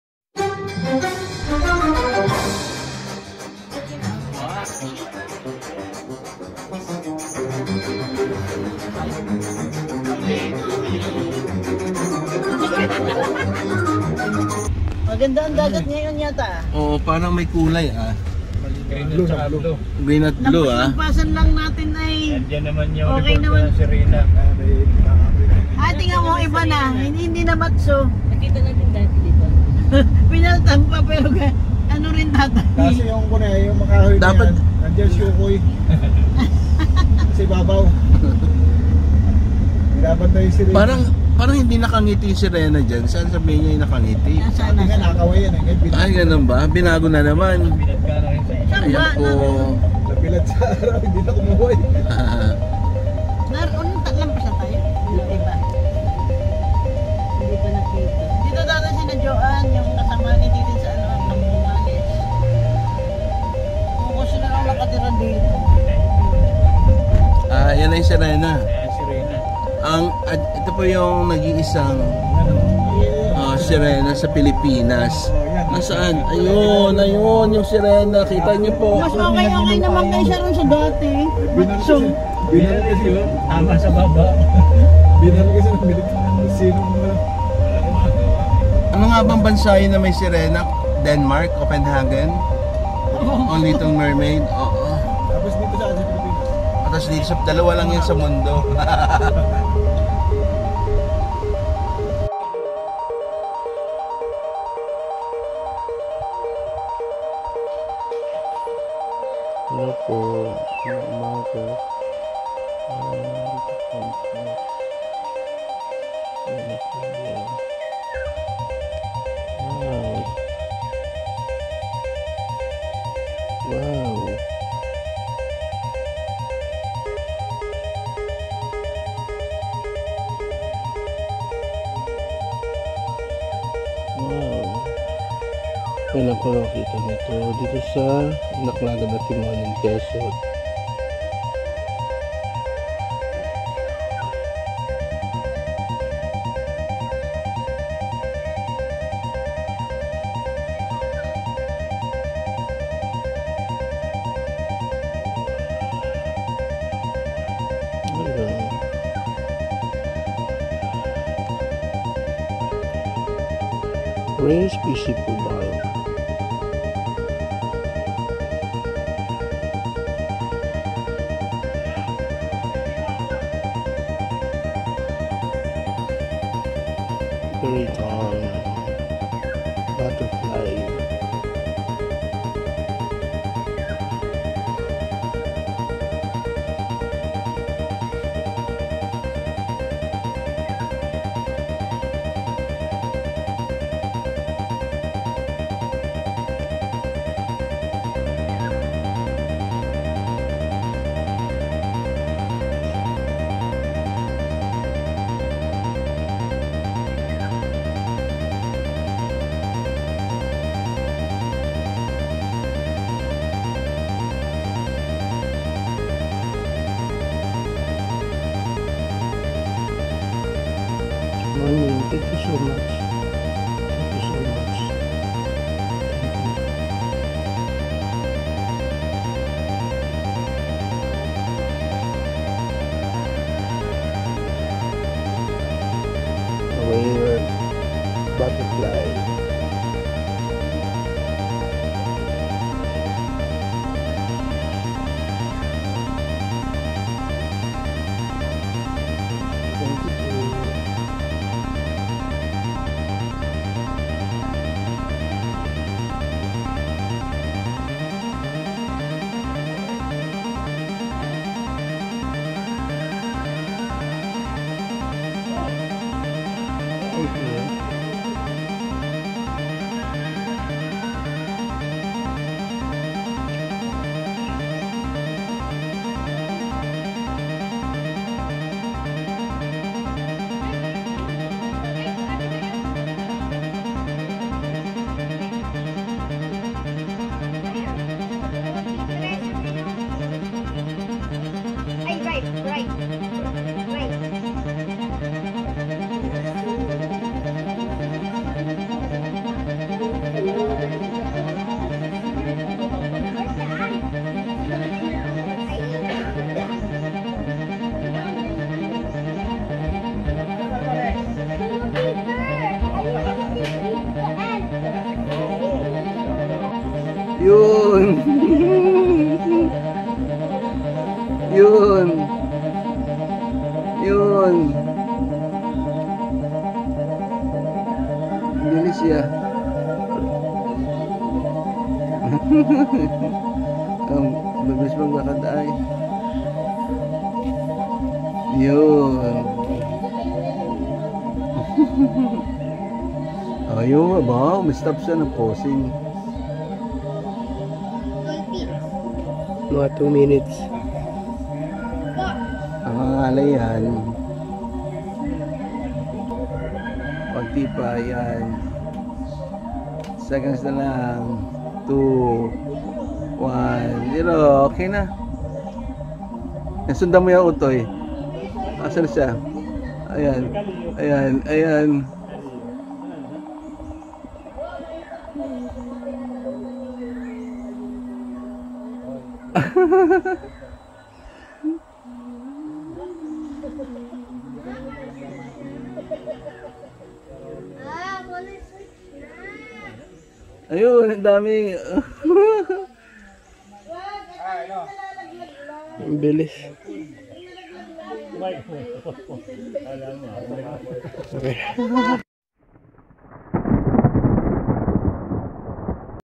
SIRENA, MAHIWAGANG PARU-PARO' MALILIKOT NA GUYAM NG ATIMONAN
Maraming kakaibang enkwentro ang aming naranasan sa bayan ng Atimonan, Quezon. Kabilang ang batong sirena, puru-paro at mga guyam...